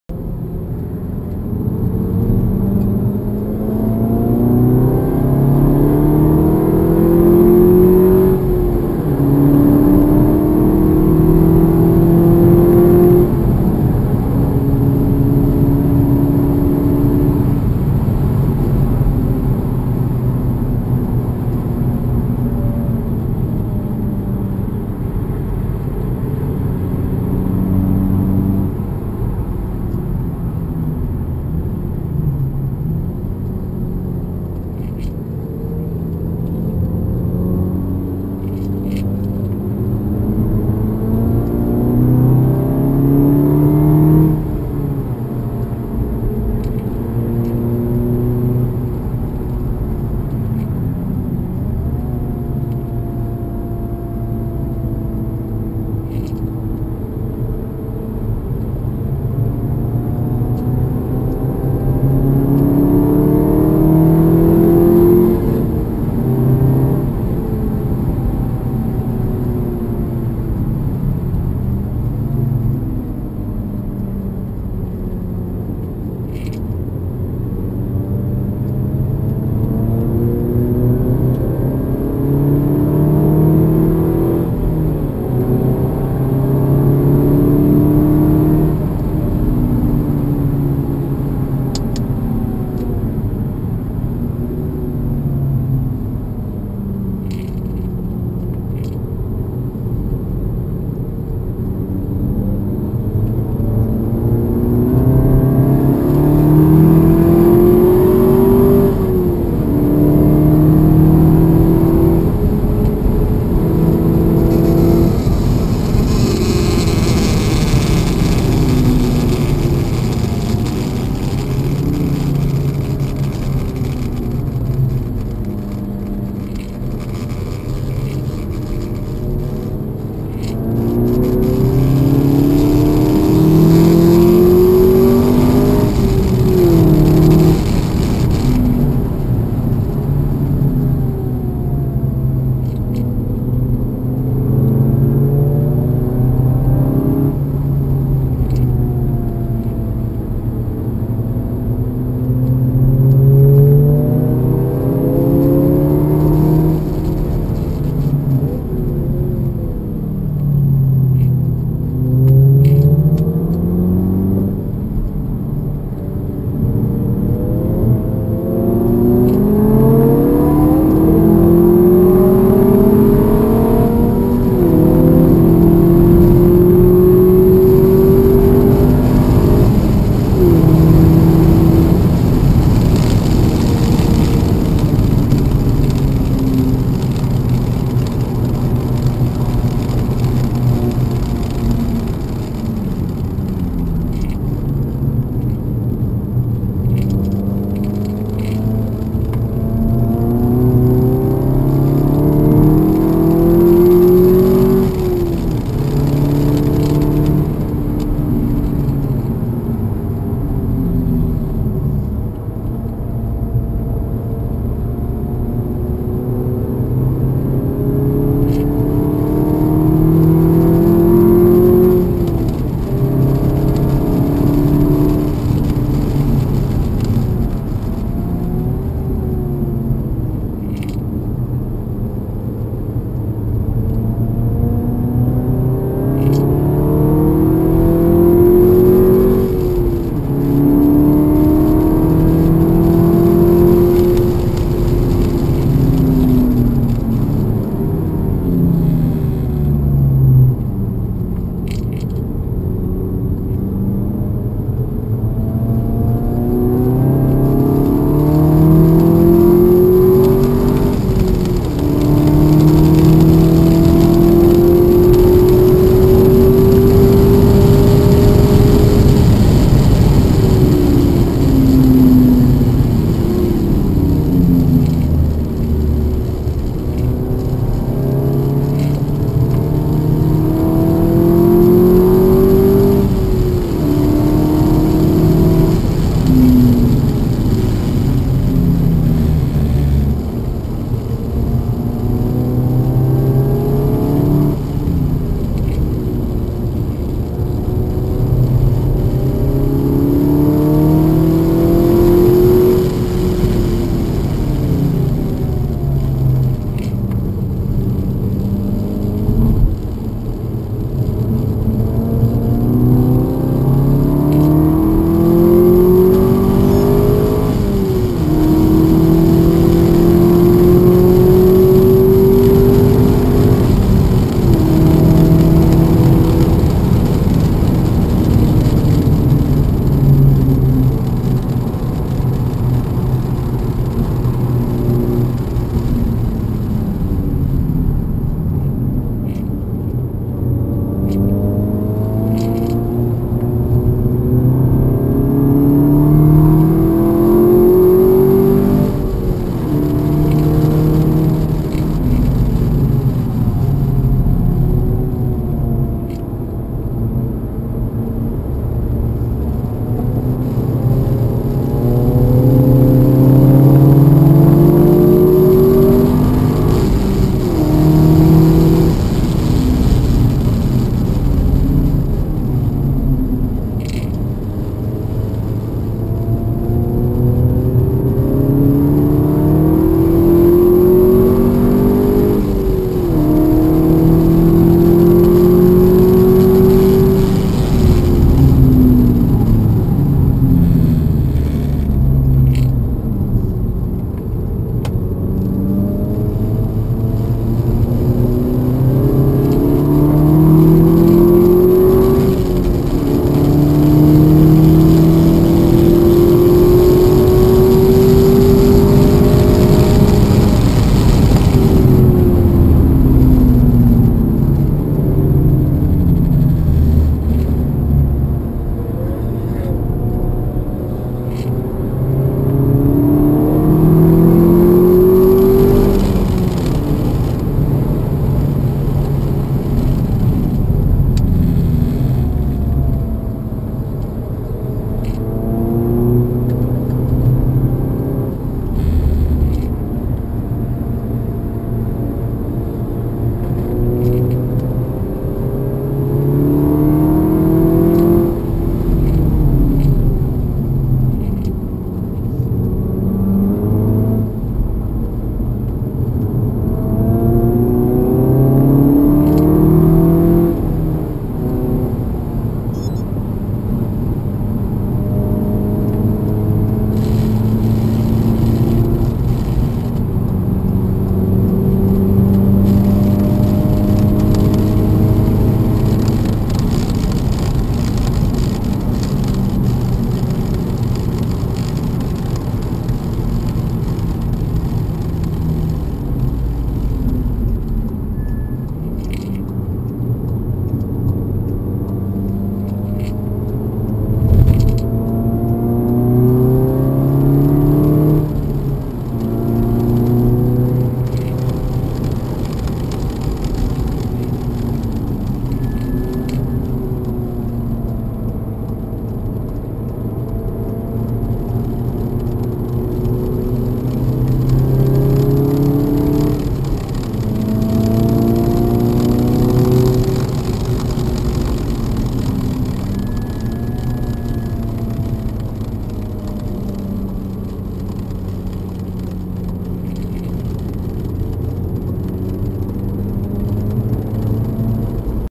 Téléthon Fast Rennes 08/12/2018 Seat Leon Cupra circuit Bugatti 09h00
Journée organisée par Fast Rennes pour le Téléthon au Bugatti (Le Mans) le 08/12/2018
Moteur 280ch stock / Disques Av Girodisc / Plaquettes Av DS Uno / Plaquettes Ar Hawk HP+ / Pneus Michelin PS4S